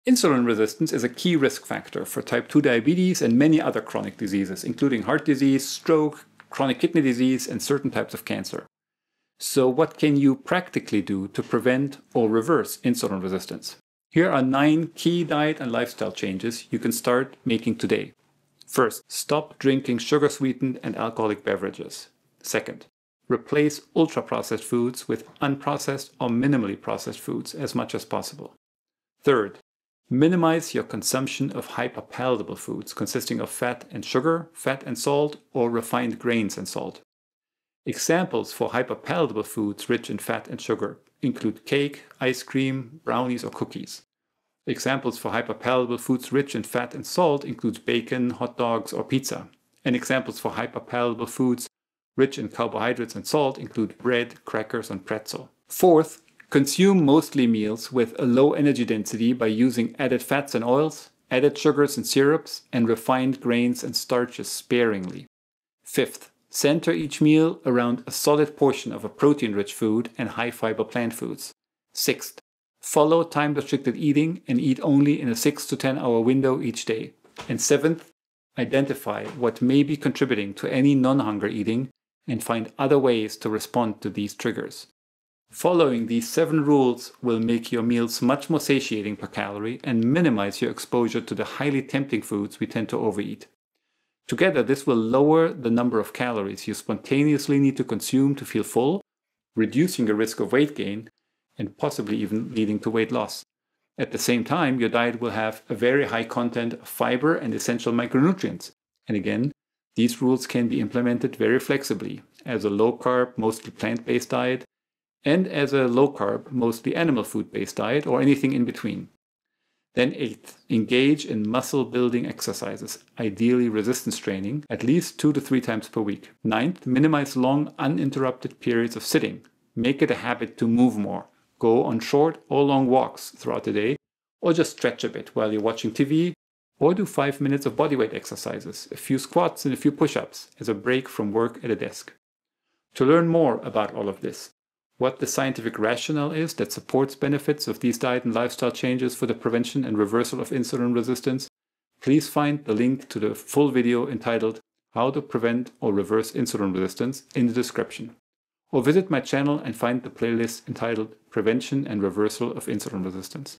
How to Prevent or Reverse Insulin Resistance
In this video, we discuss diet and lifestyle changes to prevent or reverse insulin resistance.